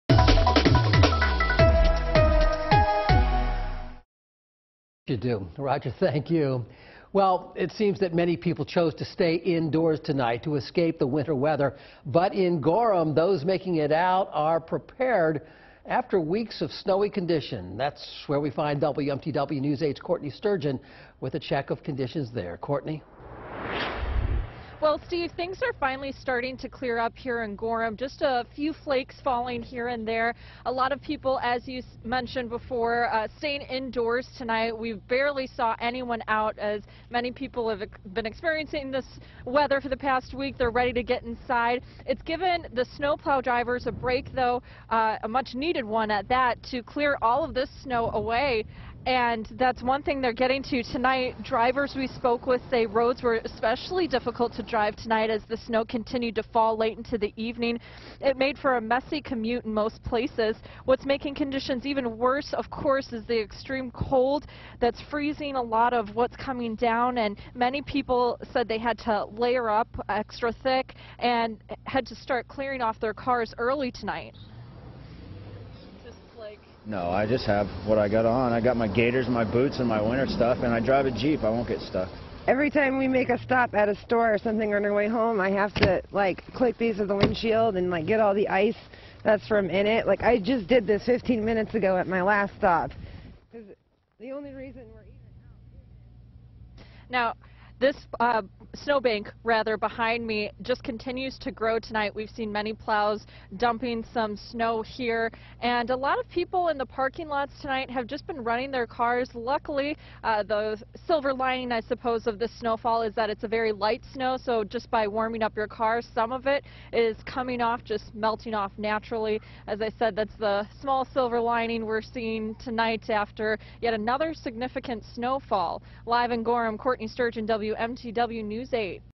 Wintry blasts keep Gorham drivers busy
WMTW News 8's Courtney Sturgeon talks to drivers in Gorham venturing out on the roads amid continued snowfall, frigid temperatures and ice buildup.